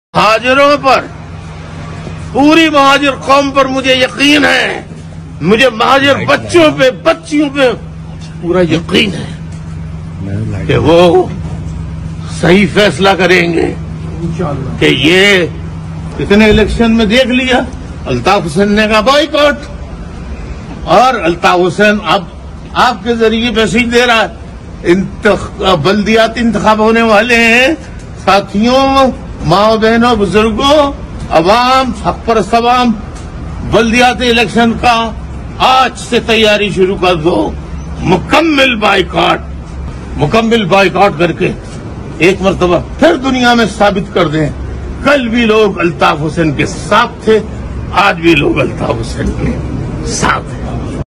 Right-wing people should start preparing for full boycott of local body elections, Altaf Hussain
مہاجر عوام اور تمام حق پرست عوام بلدیاتی انتخابات کے بھرپور بائیکاٹ کی تیاریاں شروع کردیں,الطاف حسین